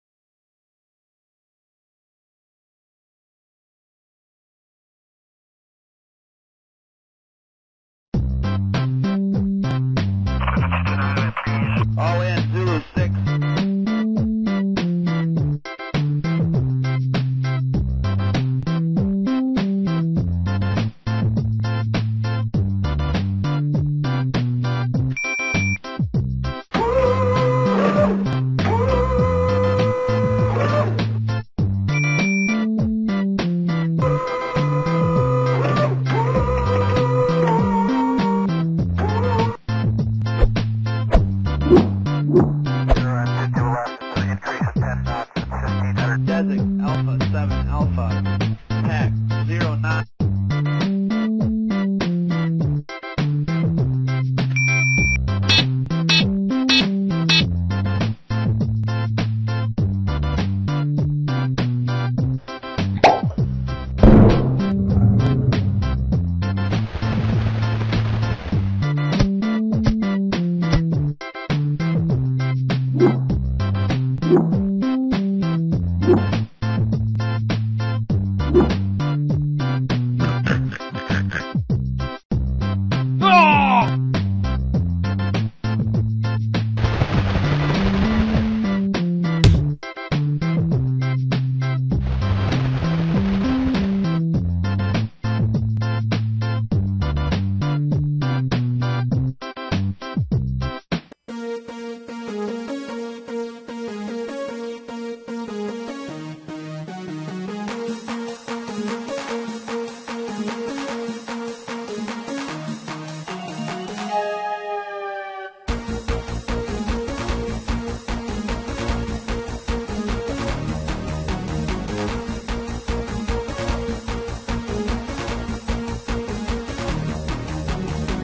A bird and a soldier
Created with Cinema 4D R 11.5
A short story about a bird and a soldier.